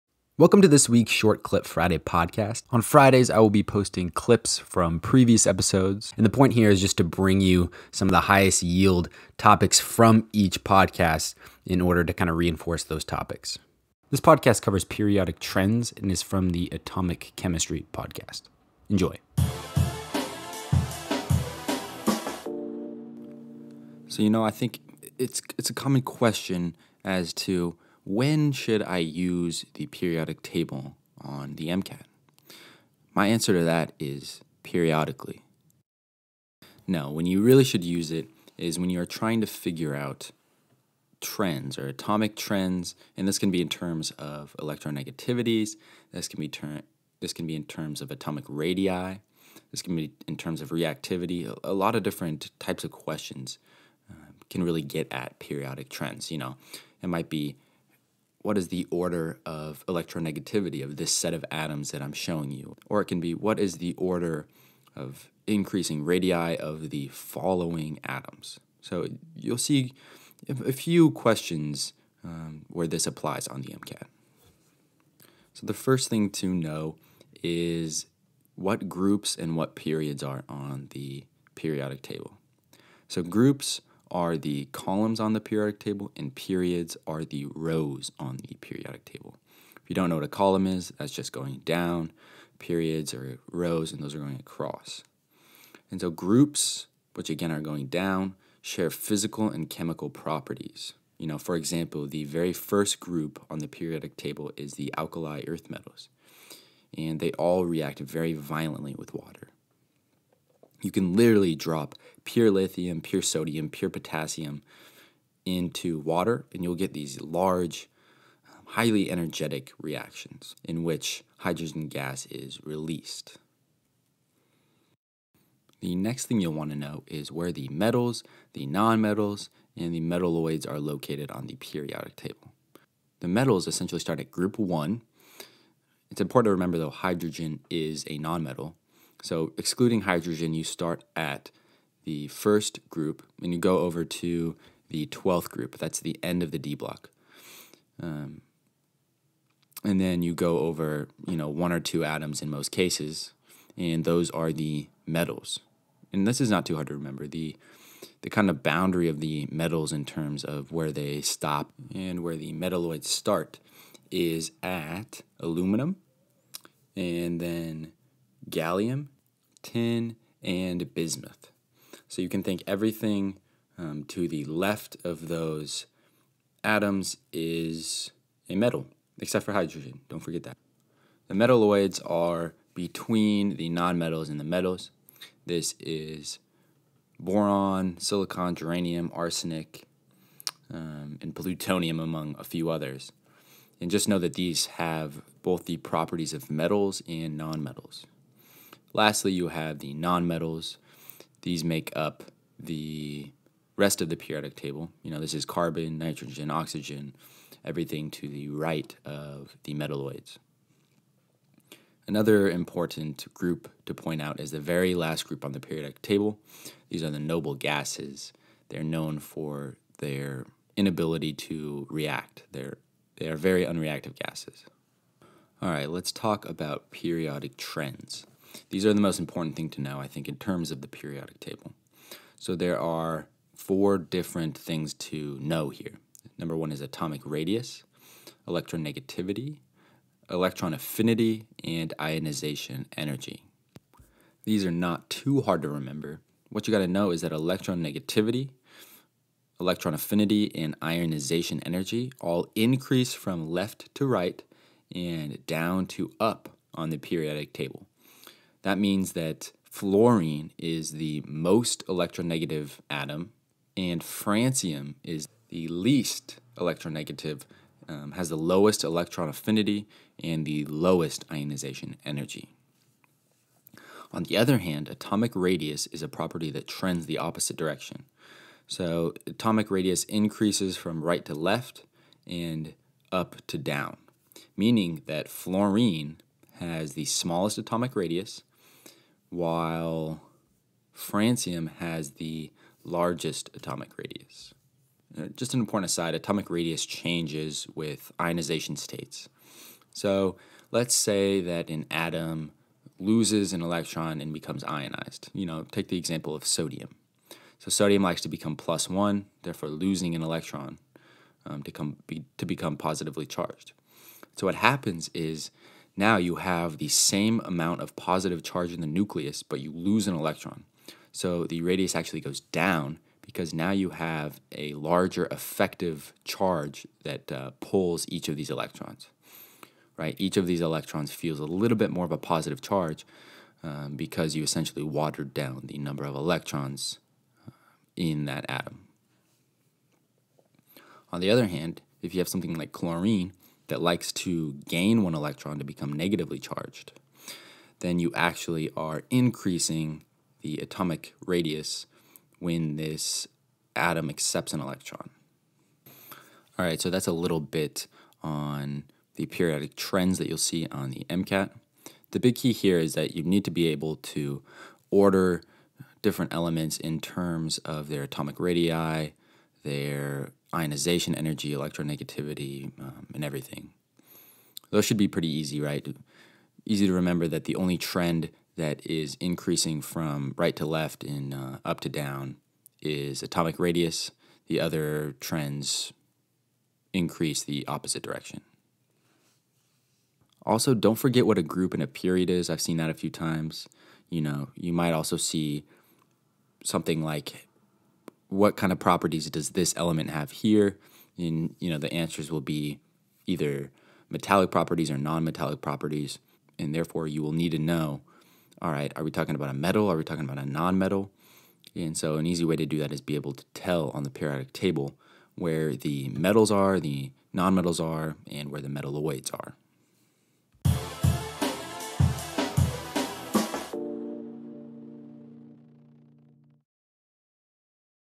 Highlight #11 - Periodic Trends
This MCAT podcast is a short segment from the "Atomic Chemistry" podcast.